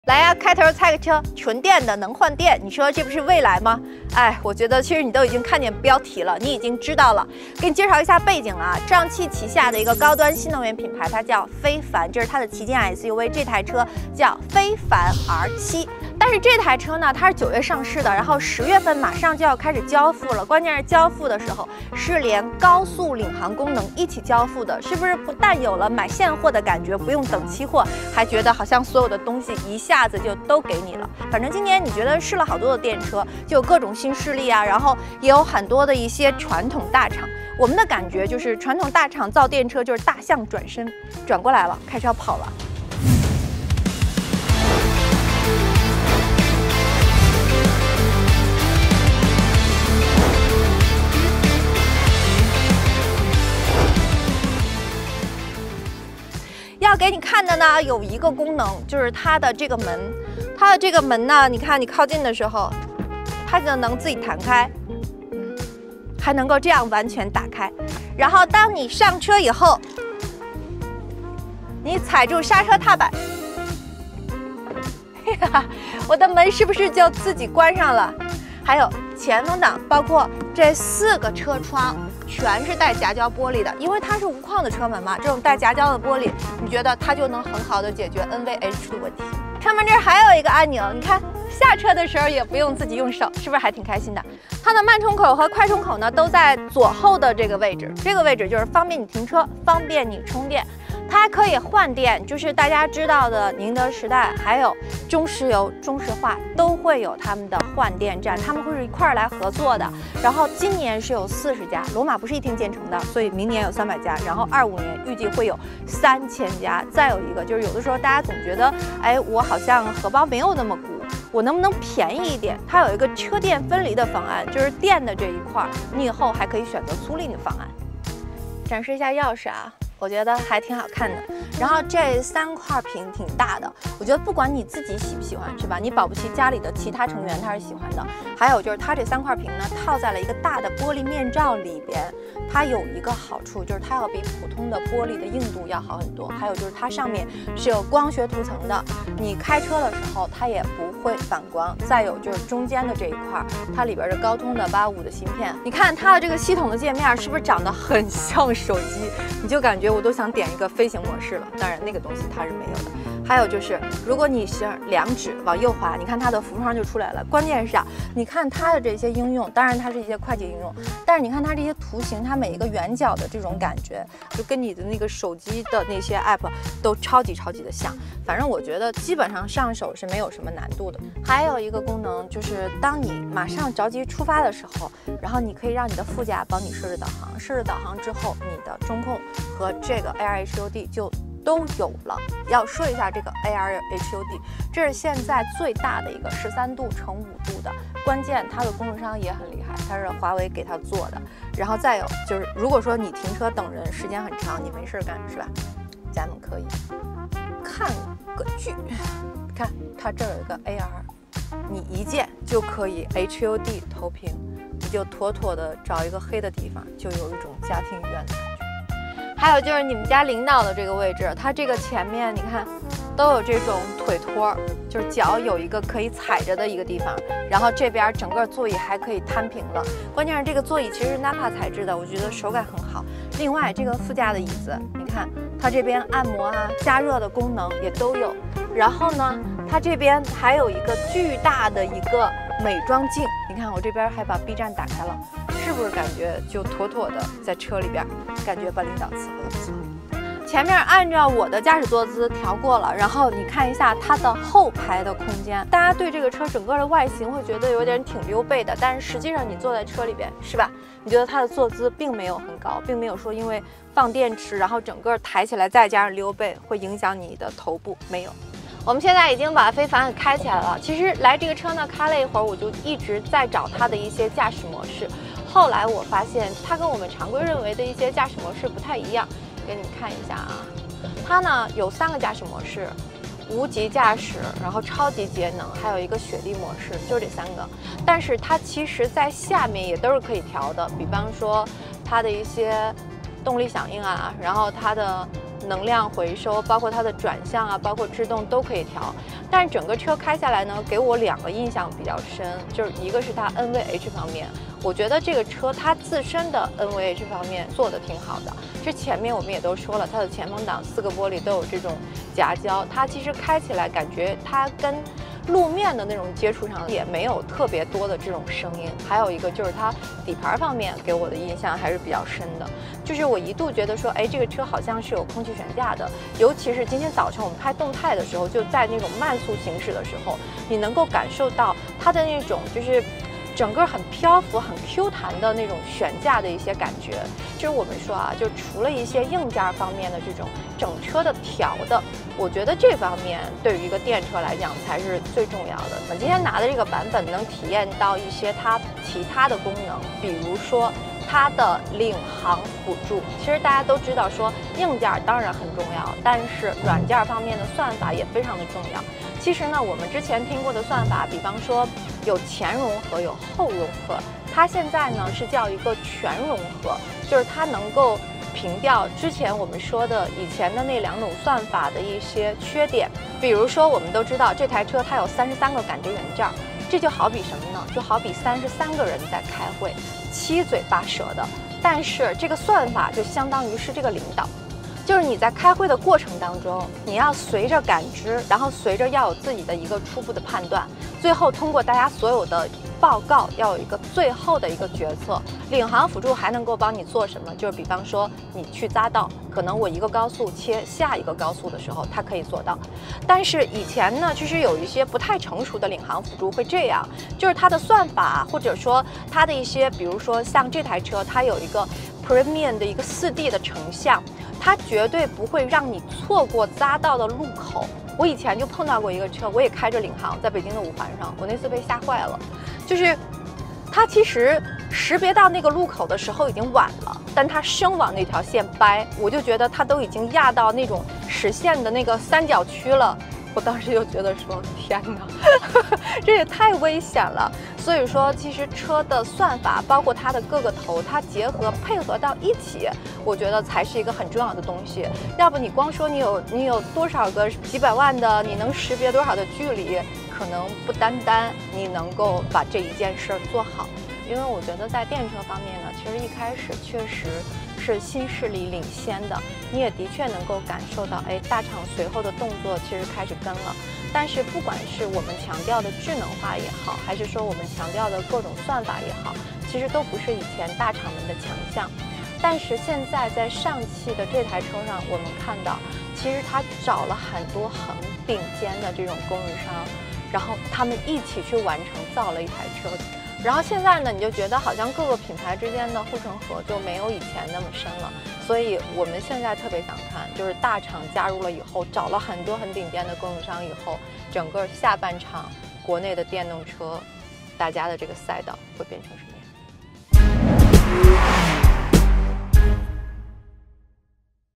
【4K】初晓敏 | 飞凡R7 大厂“新势力” 试驾飞凡R7【车若初见】
造电车，新势力开了头，传统大厂是跟着跑的状态。现在这条赛道挤满了选手，起步慢了怎么追？各有各的招，这次看飞凡R7
你的留言、按赞和分享都是我最大的动力，感谢支持！

和你分享有趣、新鲜和汽车有关或无关的视频。
微博、A站搜索“车若初见”